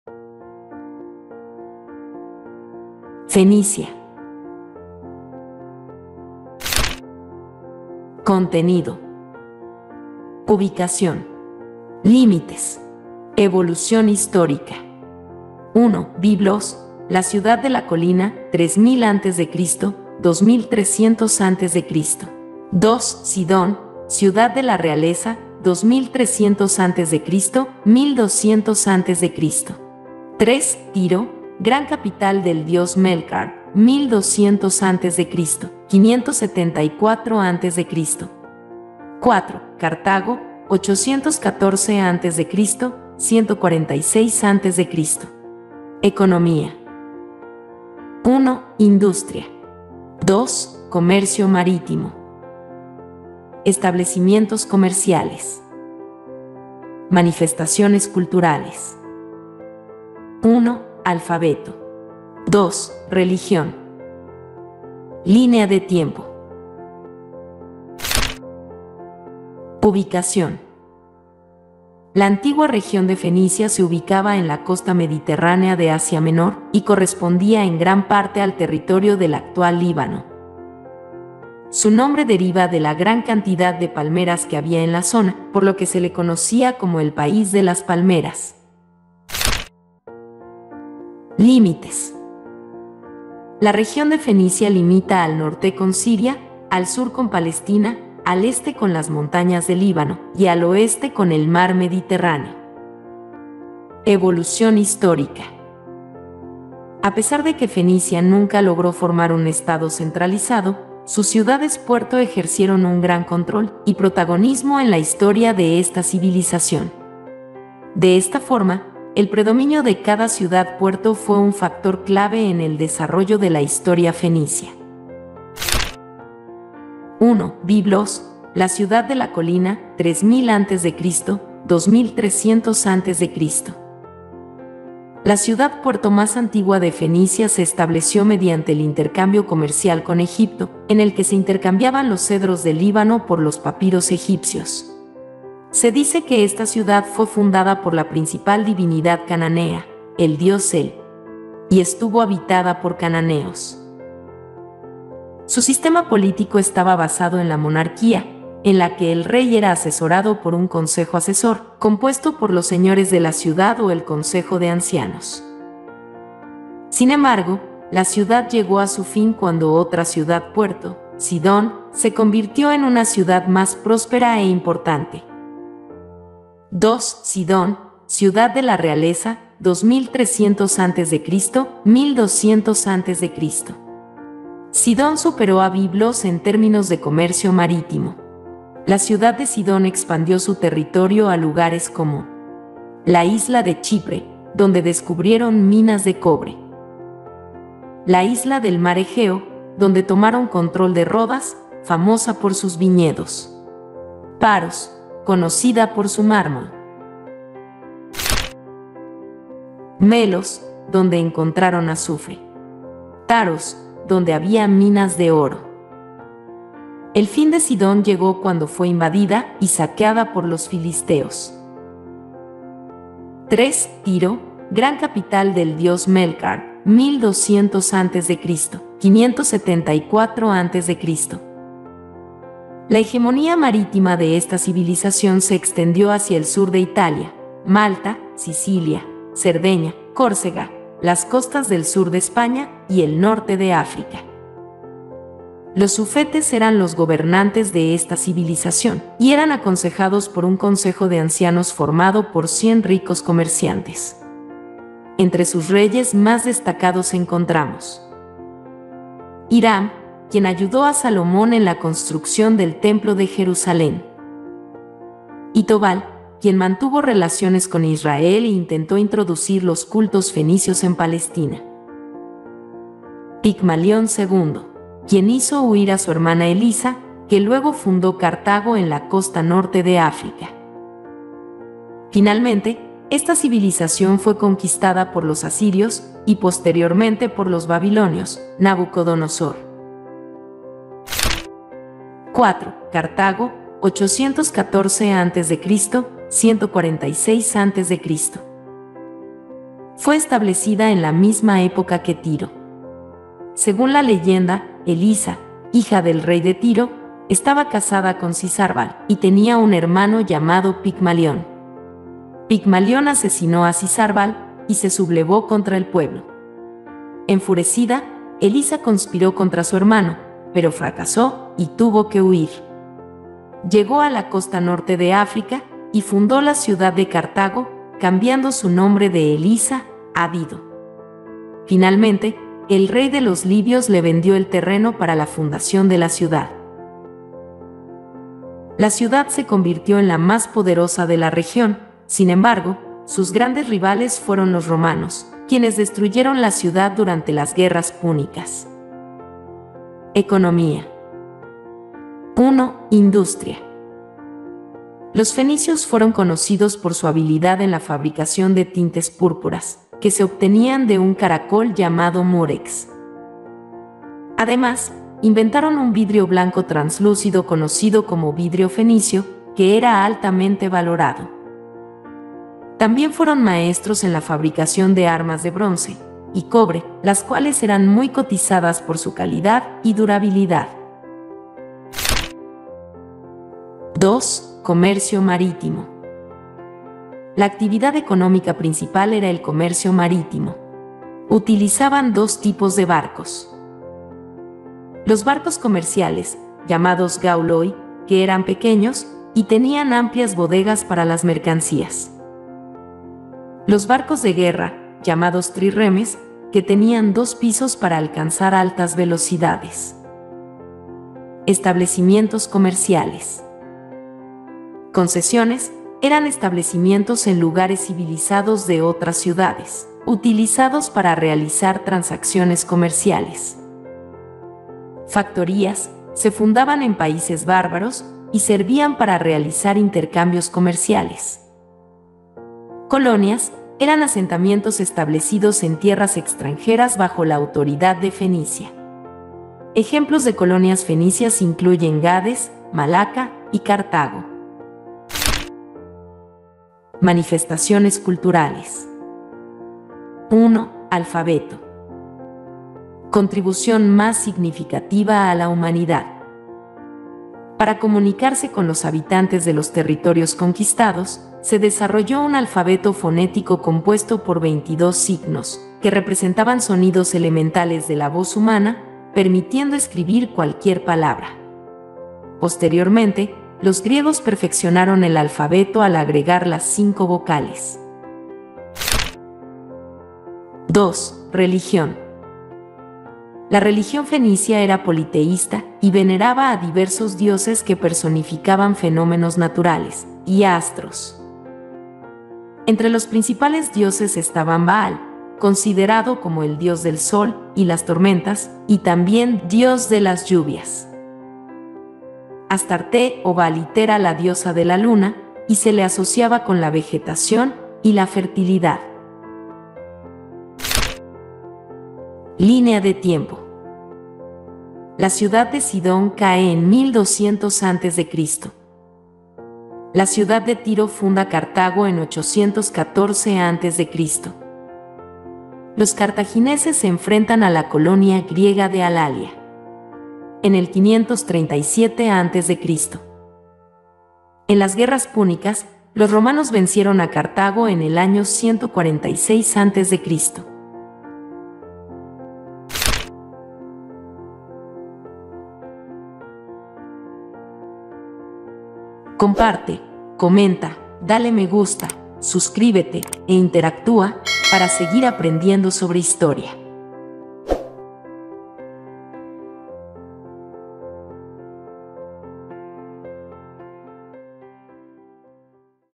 La cultura FENICIA: Comerciantes y exploradores del Mediterráneo podcast ⭐aulamedia Historia
La fascinante cultura fenicia, una civilización antigua que floreció en el Mediterráneo entre los siglos XVIII a.C. y VI a.C. A, descubrirás cómo los fenicios desarrollaron su lengua, su escritura alfabética y su sistema de comercio marítimo, que los convirtió en unos de los navegantes más habilidosos de la época. Además, conocerás sobre la religión, la sociedad, sus dioses, y su relaciones que los fenicios mantuvieron con otros pueblos del Mediterráneo, incluyendo los griegos y los cartagineses.
LA PREGUNTA🧡😊
Hola Comunidad : Luego de ver el video responde la encuesta para evaluar lo aprendido, en este enlace 👇

🕑marcas de TIEMPO
00:00 Intro
00:08 Contenido
01:09 UBICACIÓN
01:31 LÍMITES
01:44 EVOLUCIÓN HISTÓRICA
02:06 Biblos, «La ciudad de la colina»
02:56 Sidón, «Ciudad de la realeza»
03:43 Tiro, «Gran capital del Dios Melkart»
04:52 Cartago
05:50 ECONOMÏA
05:51 Industria
06:24 Comercio marítimo
06:51 ESTABLECIMIENTOS COMERCIALES
07:27 MANIFESTACIONES CULTURALES
07:30 Alfabeto
08:00 Religión
08:32 LÍNEA DE TIEMPO
☟ ¡SÍGUENOS AQUÍ! ☟